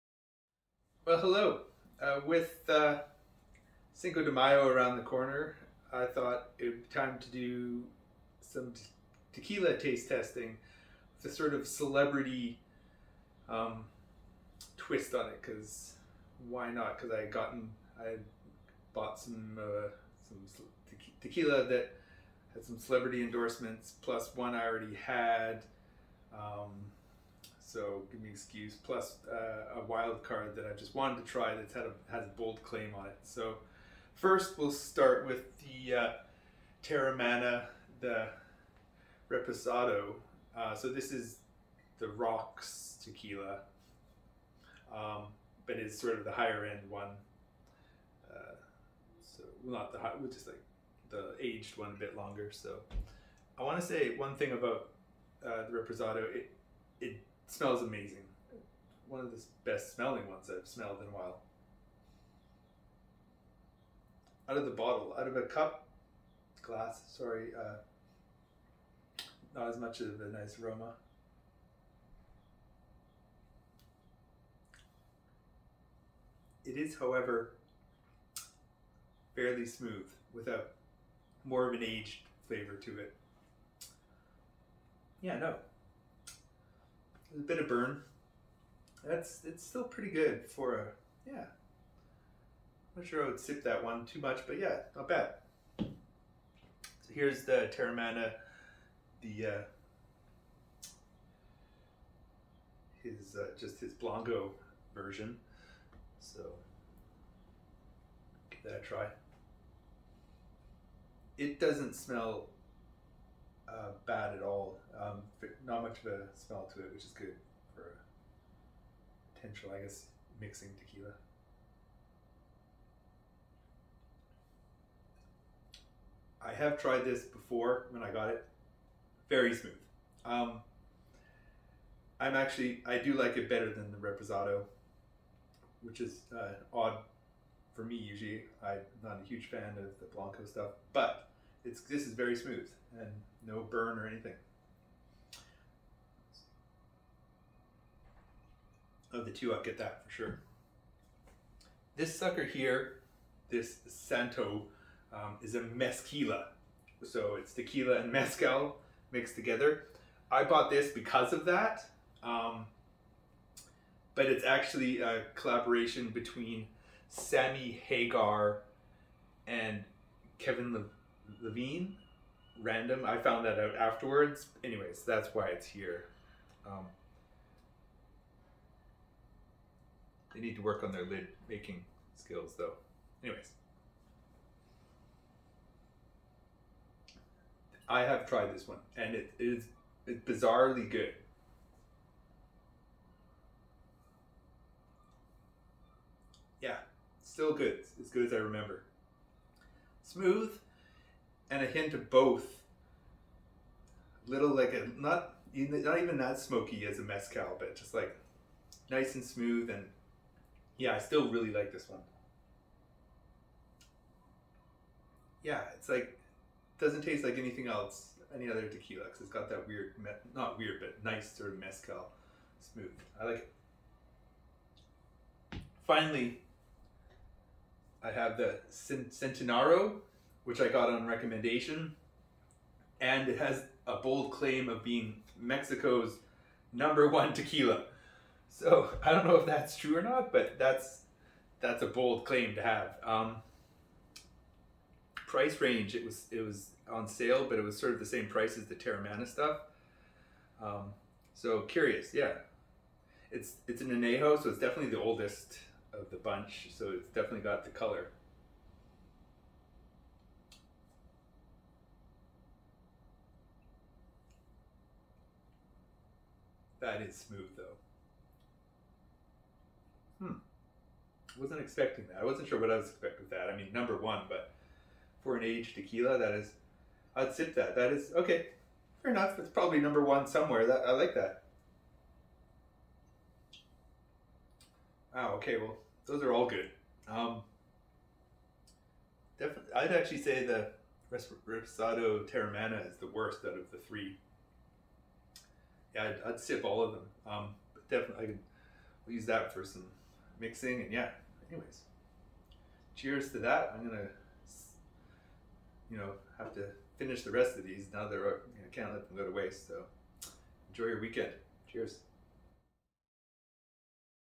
Celebrity Tequila Tasting! - Teremana & Santo Mezquila -
With Cinco de Mayo just around the corner its time to test out some new Tequila's with a celebrity twist.

Well Teremana is new here in Canada at least...

- Teremana Blanco Tequila
- Teremana Reposado Tequila

As always zero production value and quality control guaranteed!